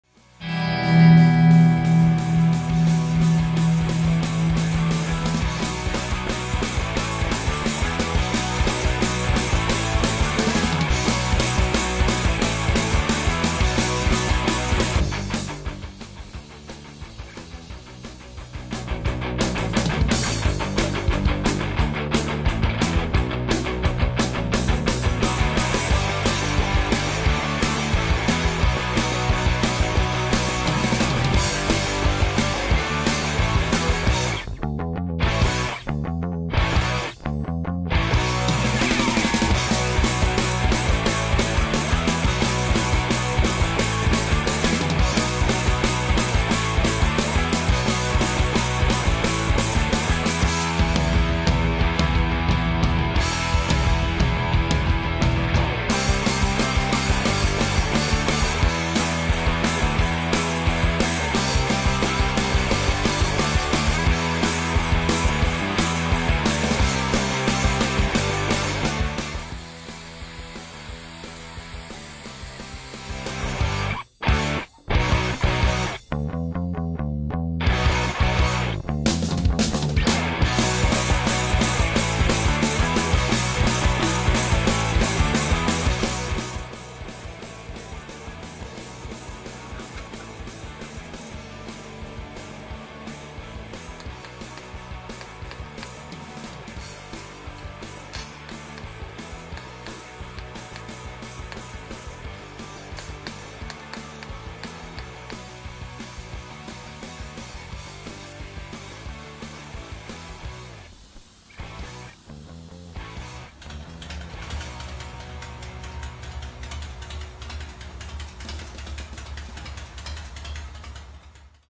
커스텀바이크 빌드 (Make Oil Tanks Mount) / 쉐이크피스톤 MC
빈티지 할리데이비슨 오일탱크 마운트 커스텀 제작 VLOG
(쉐이크피스톤 모토사이클)